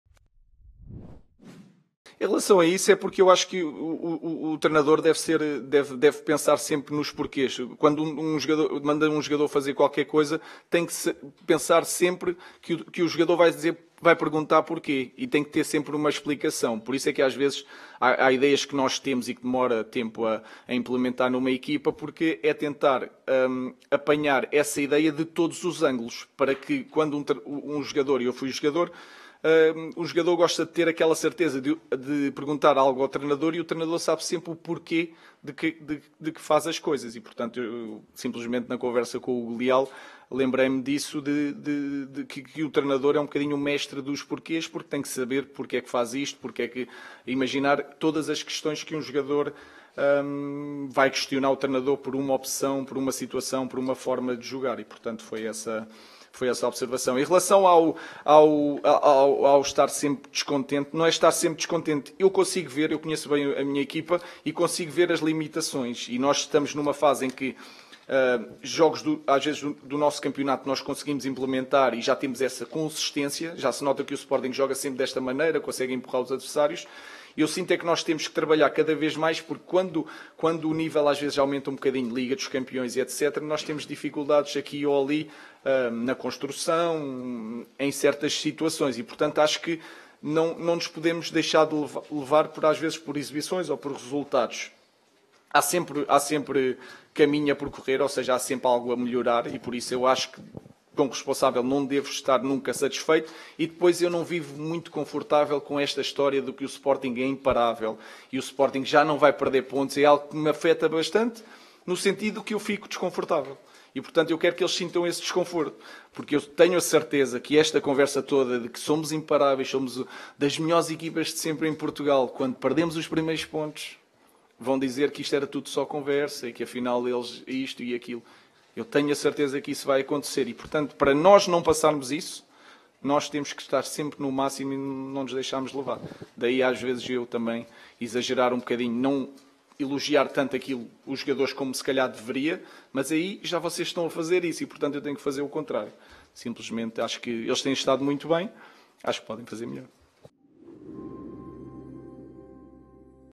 "Eles têm estado muito bem mas podem fazer melhor" | Rúben Amorim sobre o seu modelo de jogo
Rúben Amorim em antevisão ao jogo contra o Estoril da 7ª jornada do campeonato.